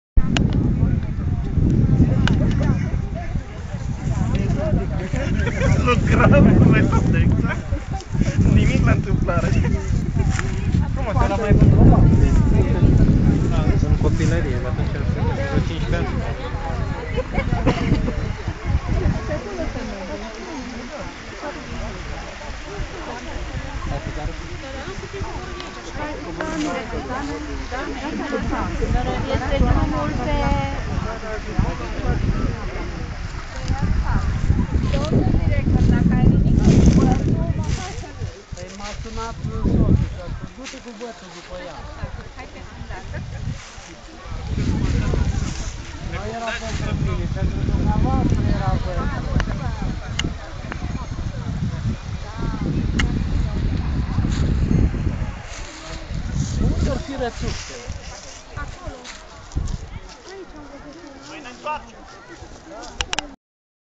Bibliotecari participanti la Conferinta ANBPR vizitand Lacu Rosu
Lacu Rosu - Harghita, 29-31 oct. 2009, cea de-a XX-a Conferinta Nationala a ANBPR; intr-o binemeritata iesire in natura, bibliotecarii viziteaza Lacu Rosu.